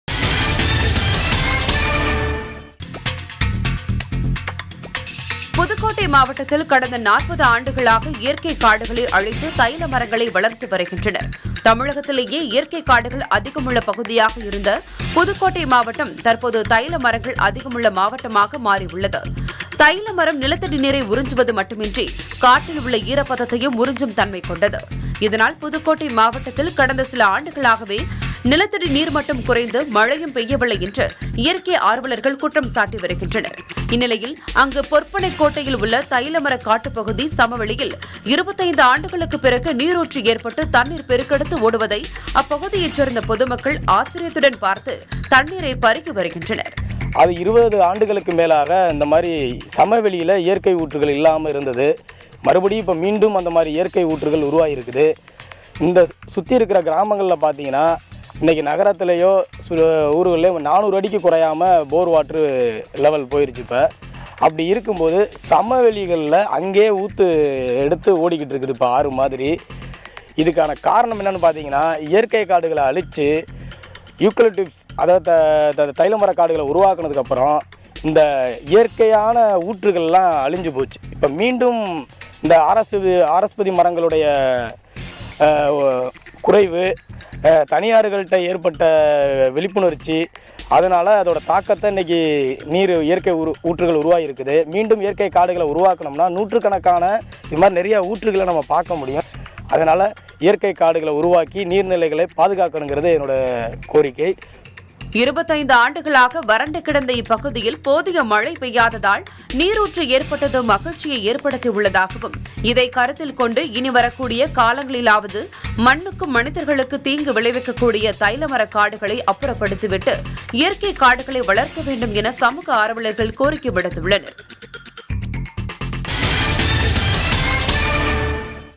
25 ஆண்டுகளுக்குப் பிறகு தைல மரக் காட்டில் நீரூற்று! புதுக்கோட்டை | செய்தித் துளிகள்
25 ஆண்டுகளுக்குப் பிறகு  தைல மரக் காட்டில் நீரூற்று! புதுக்கோட்டை | செய்தித் துளிகள்